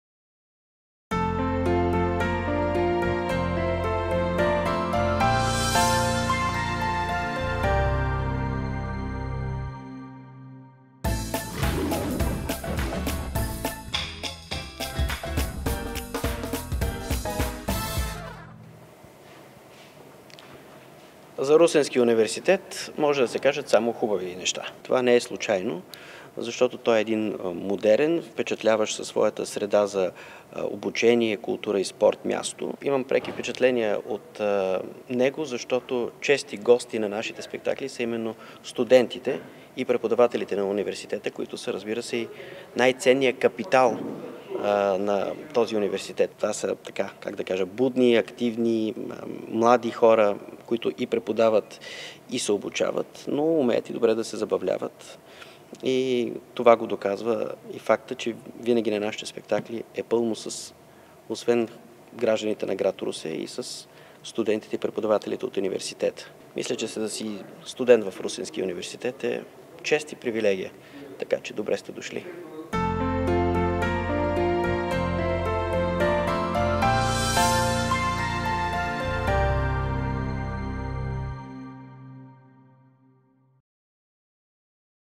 Мариан Бачев за Русенския университет
Мариан Бачев, актьор, за Русенския университет от първо лице:„Да си студент в Русенския университет е чест и привилегия“.